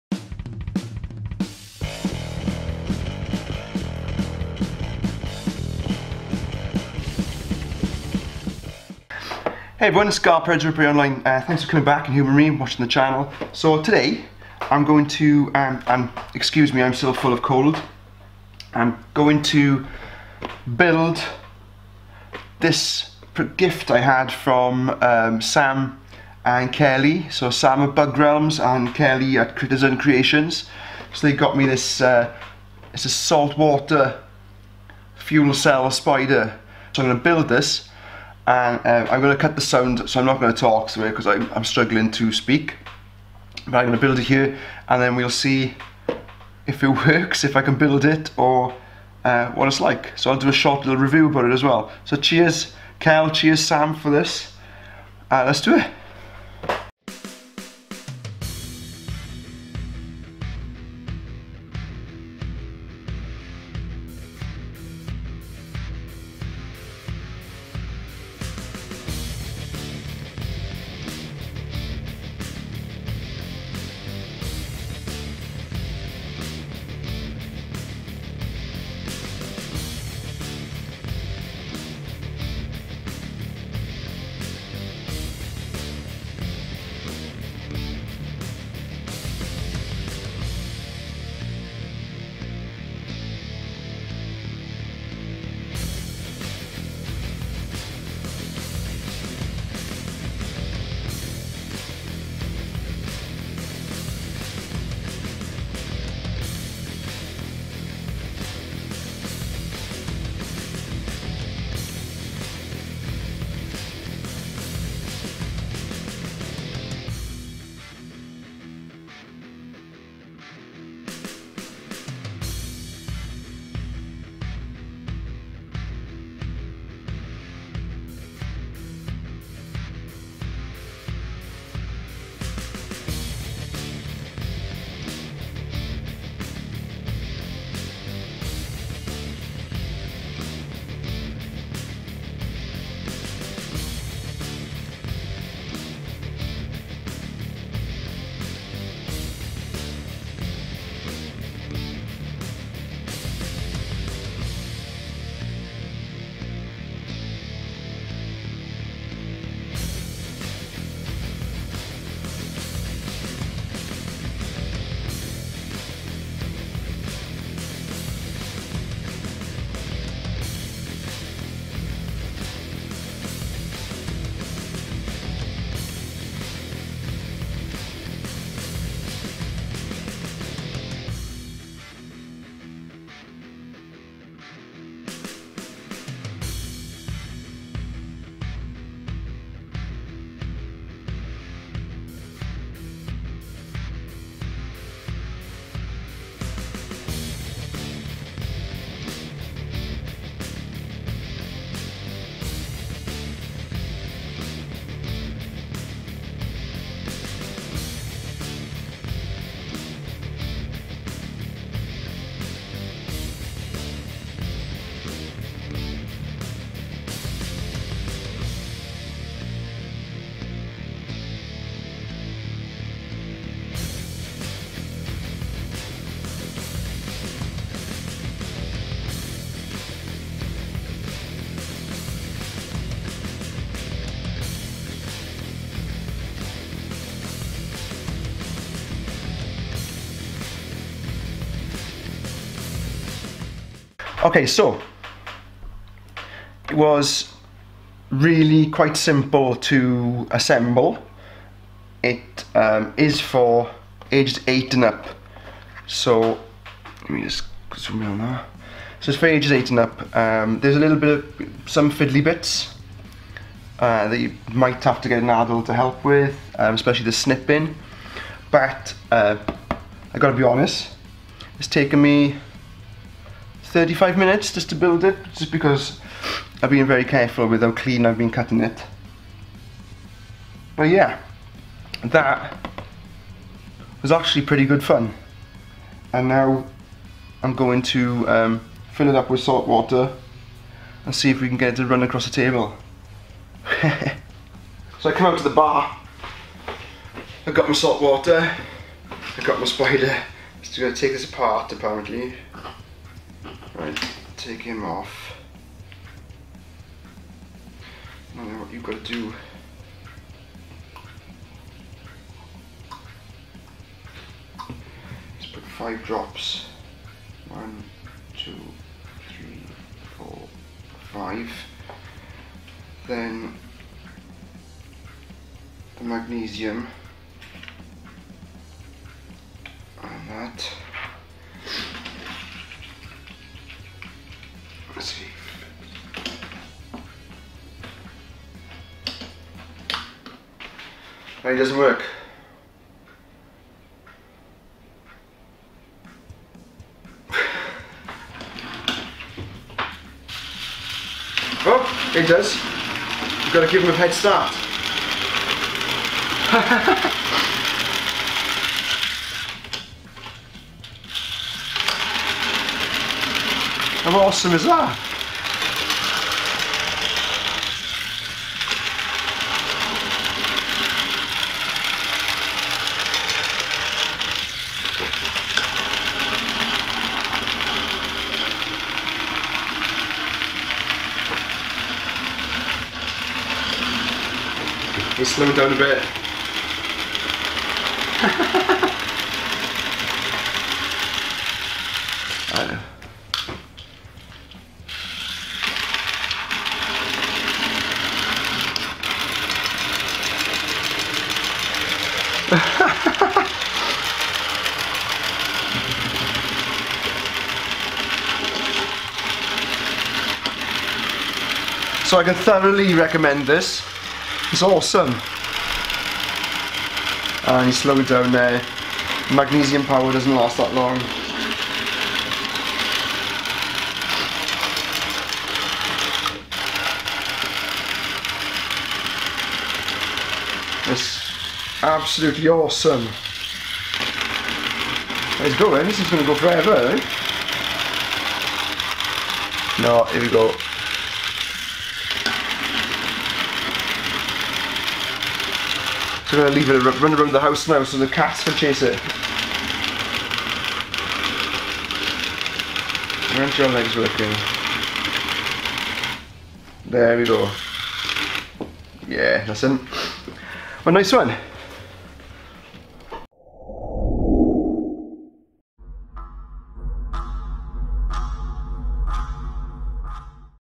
Salt Water Fuel Cell Spider - building a kiddies toy (it's pretty cool)
Now and again there are comps to win spiders

Credits :
Thank you to Funky Bass Magician Ricky Cocozza.

Thanks to Claire Cocozza (Instagram @clairec3003) - I couldn't do any of this without you